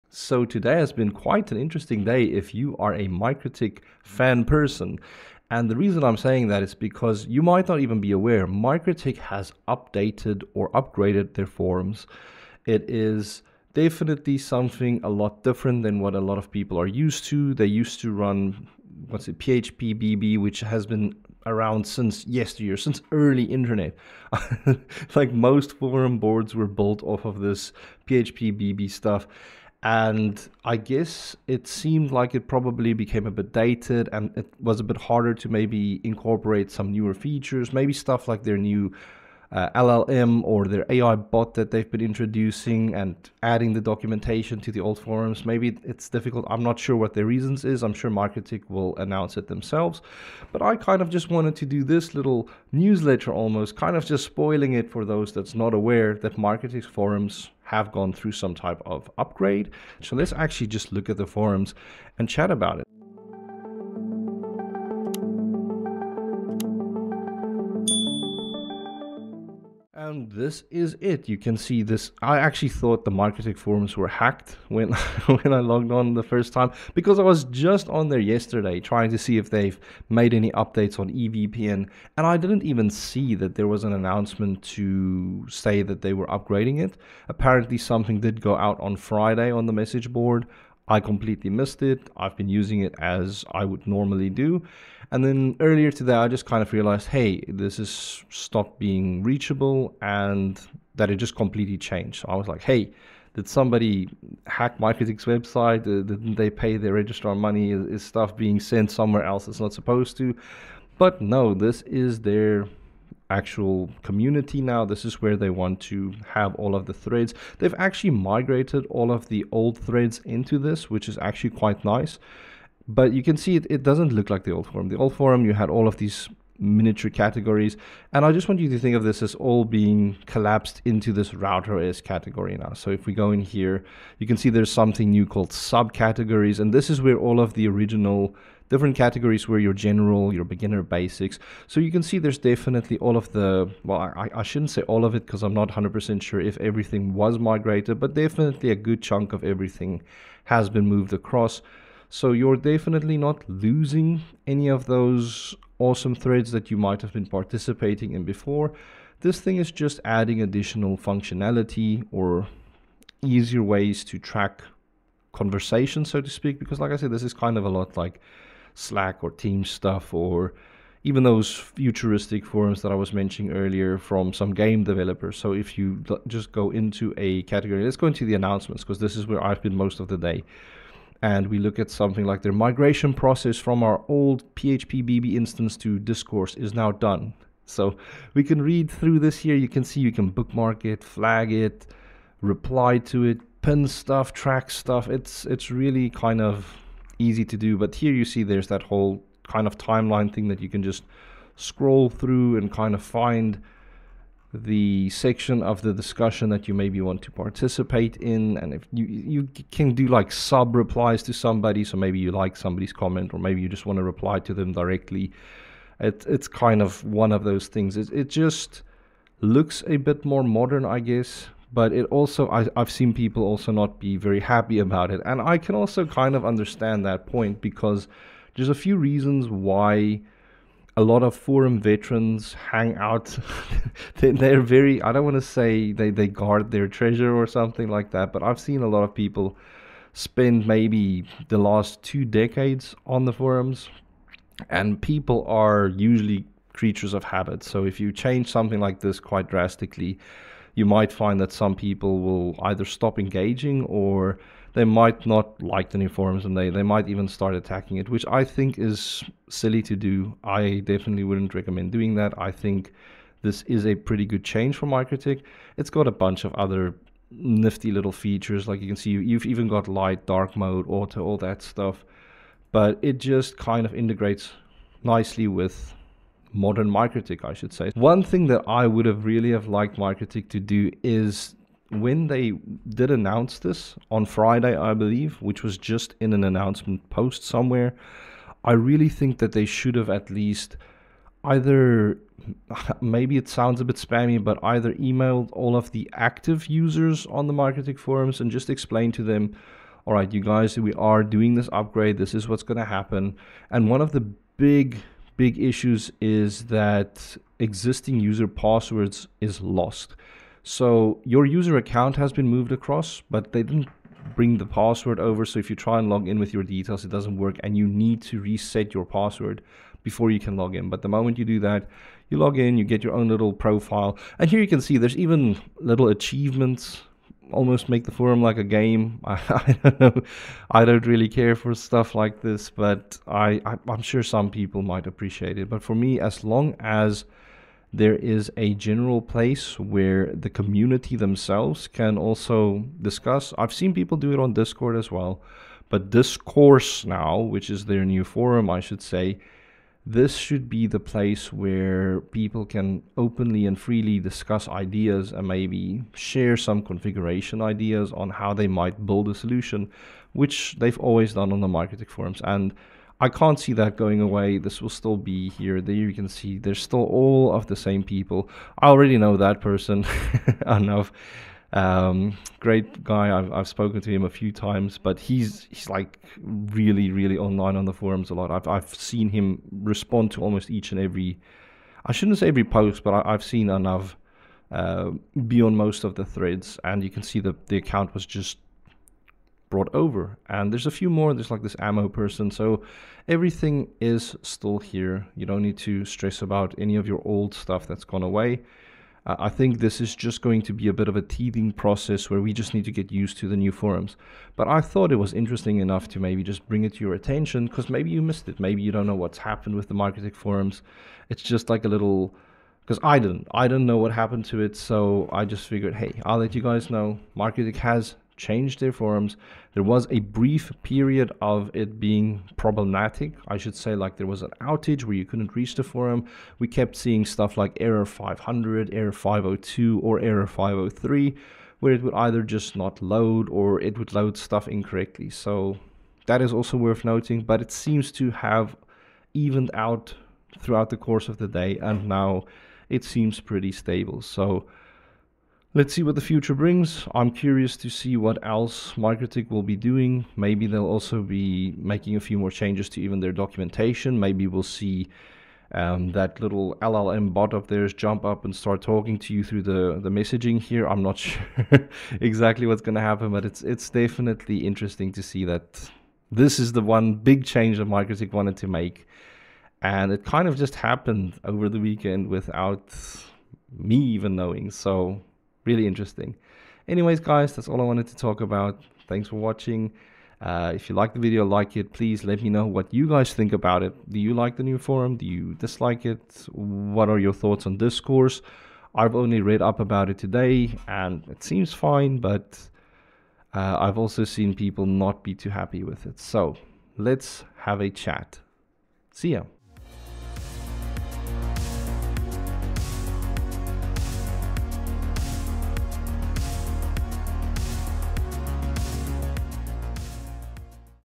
MikroTik Forums Just Got a Big Makeover!
MikroTik has finally done it, they have gotten rid of their early 2000s forum and updated to a more modern one called Discourse!

📕Chapters:📕
00:00 - Introduction
01:10 - Going over the new forums
05:03 - Important Note (Reset your password)